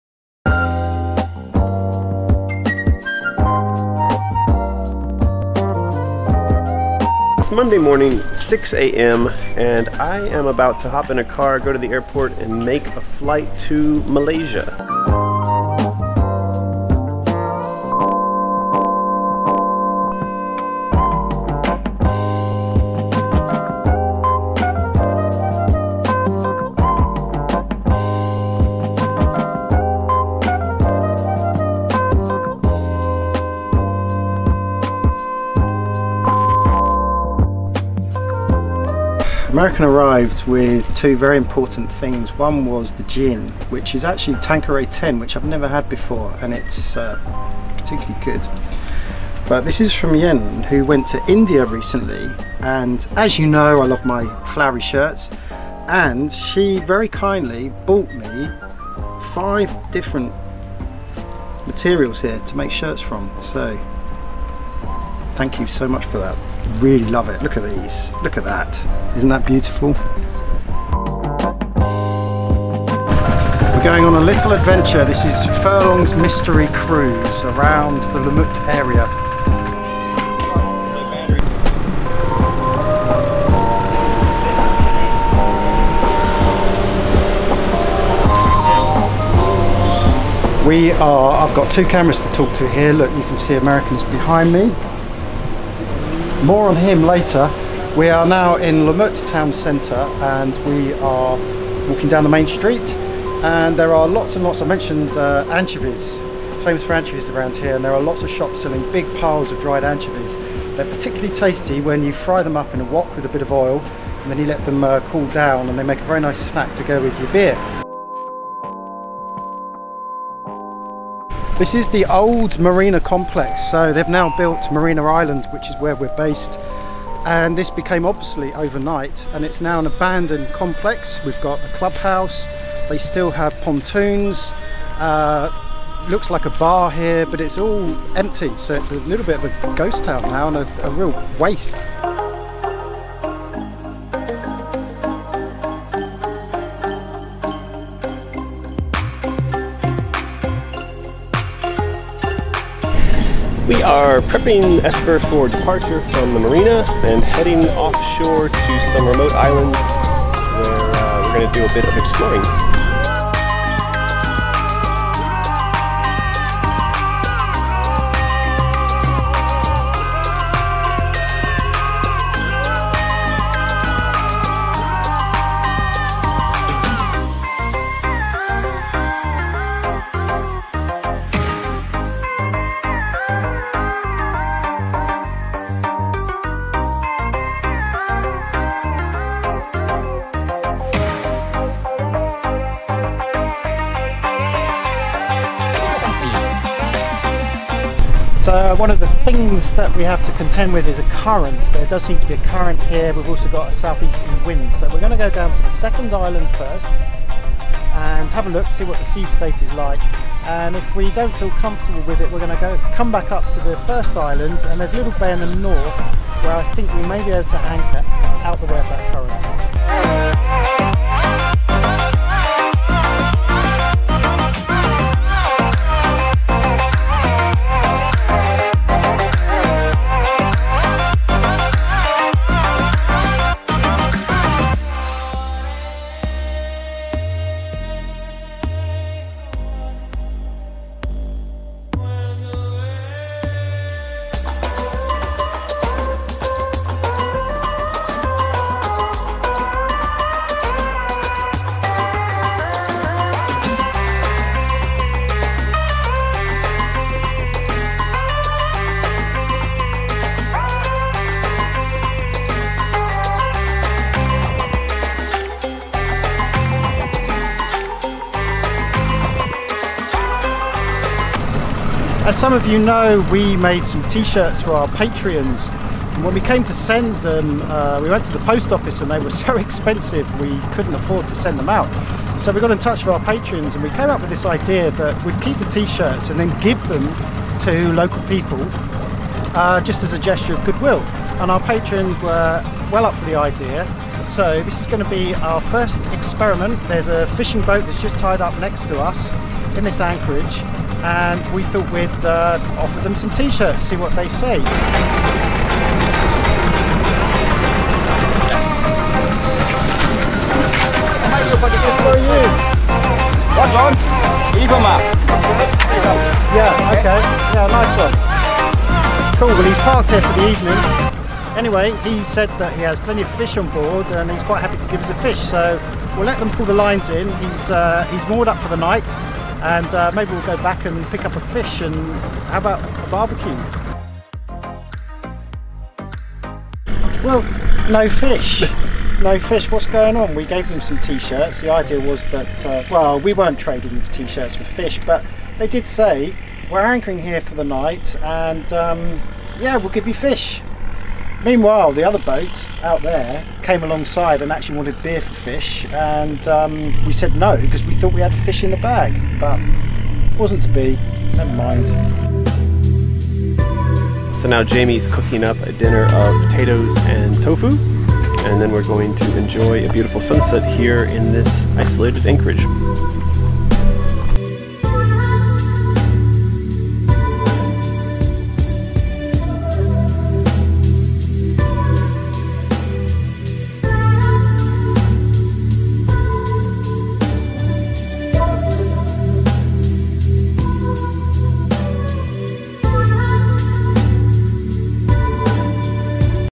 Malaysia Sailing - VLOG41
I head to Malaysia to hang out with my friend Jamie aboard Esper.